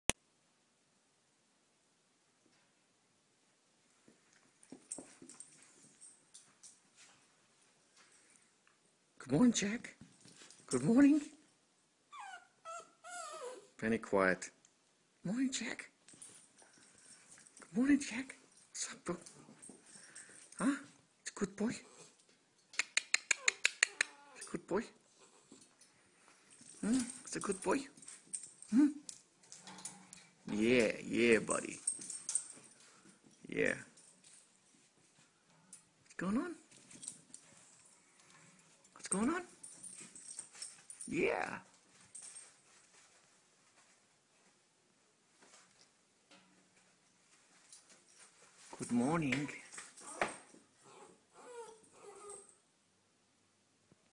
Jack the AFrican Grey parrot
We just got jack he has a lot of toys...He is 2 years old